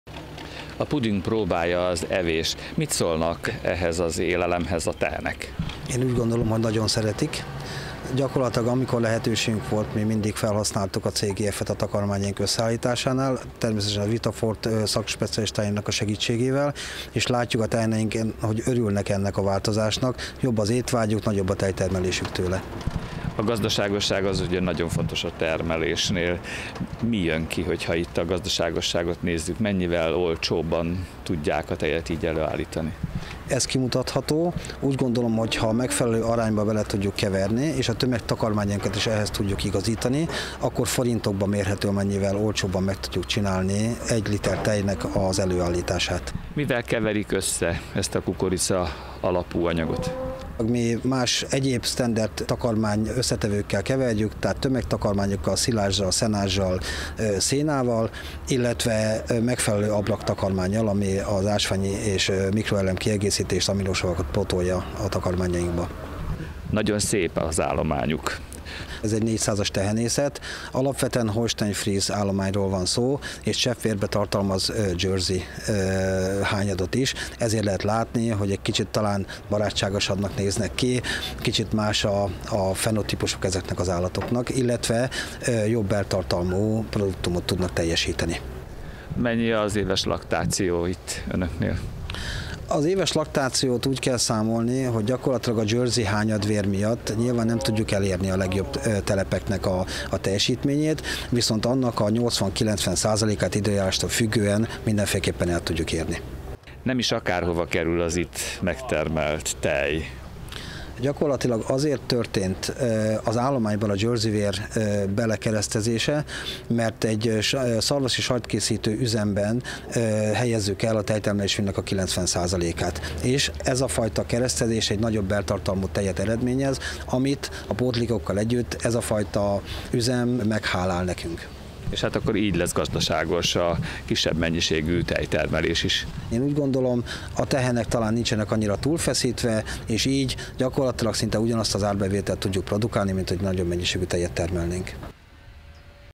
A Hungrana által gyártott CGF takarmánykiegészítőt eszik a tehenek
Szőke-Molnár Tibor - ügyvezető, Béke Agrár Kft., Orosháza